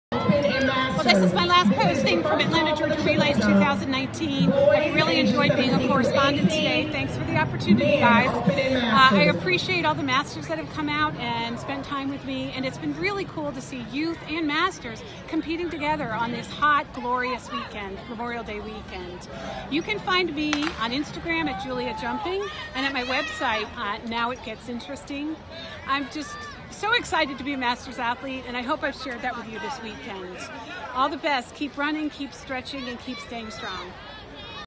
Final post from 2019 Atlanta Georgia Relays
Final wrap up of the 2019 Atlanta Georgia Relays. I was honored to be asked to be a correspondent for this exciting weekend. After three days and 40 interviews, I had very little voice left, but I hope you can hear the excitement I felt being there this weekend. It’s great to be a masters athlete and share in this community! Find all the interviews on YouTube @juliacurran. 😎
@atlantageorgiarelays2019 @usatfmasters Julia Curran